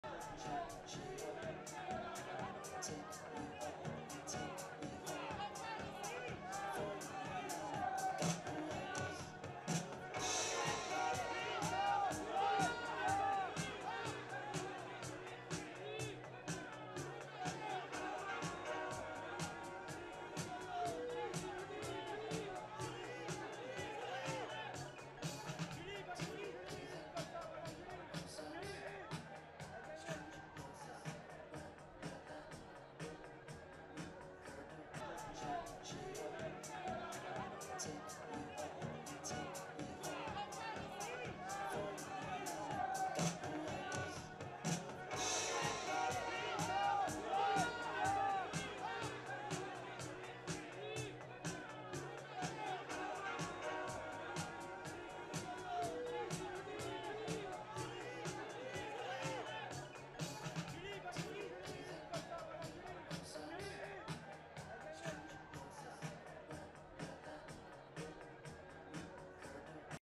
FILE:FRENCH ACTRESS SUES TABLOID OVER AFFAIR NEWS
French Actress Julie Gayet is suing the magazine over their publication her alleged affair with French President Francois Hollande.